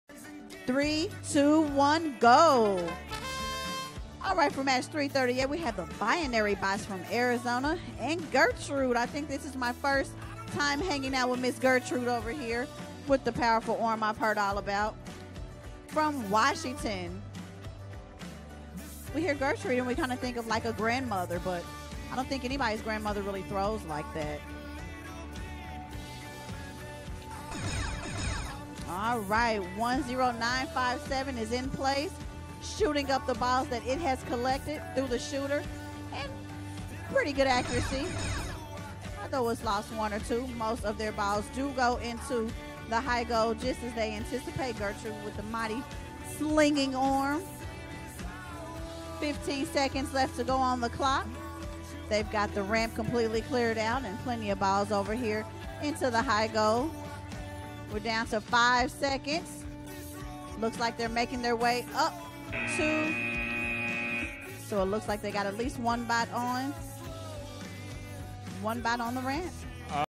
VEX Worlds 2016 - VEXIQ Middle School - Science - Qual 338 (10957 4713B) 67
VEX Worlds 2016 - VEXIQ Middle School - Science - Qual 338

Team Score 67:
10957 - Binary Bots (Imagine Avondale, Avondale, Arizona, United States)

4713B - Gertrude (Trashcan to the Left, Mukilteo, Washington, United States)